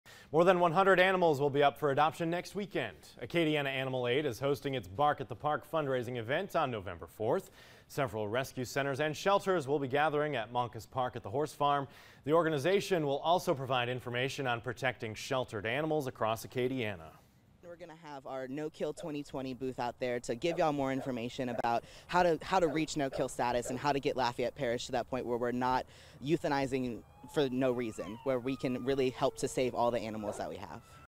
Bark in the Park set Nov 4th
More than 100 animals will be up for adoption next weekend.
Acadiana Animal Aid is hosting its Bark in the Park fundraising event on November 4.